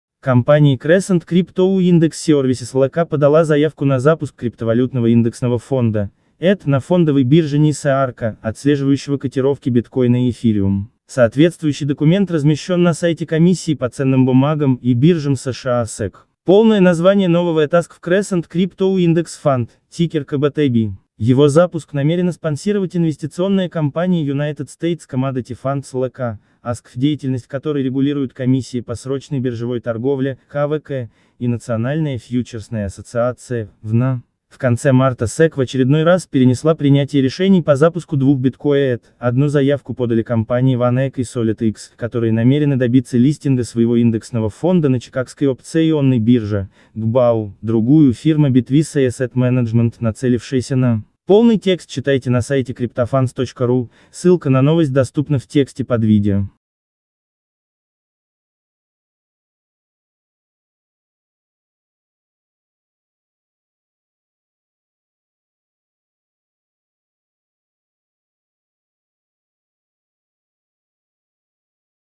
в sec направлена новая заявка на запуск криптовалютного etf
Компания Crescent Crypto Index Services LLC подала заявку на запуск криптовалютного индексного фонда (ETF) на фондовой бирже NYSE